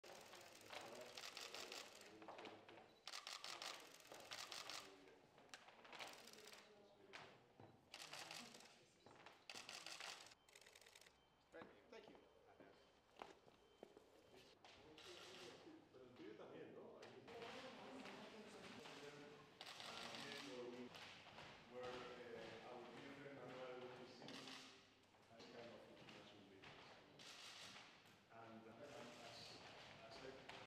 El presidente del Gobierno recibe al Consejo Internacional de IA | Recursos multimedia
Mantiene un encuentro con el Consejo Internacional de Inteligencia Artificial, en el Complejo de la Moncloa.
Asiste el ministro para la Transformación Digital y de la Función Pública.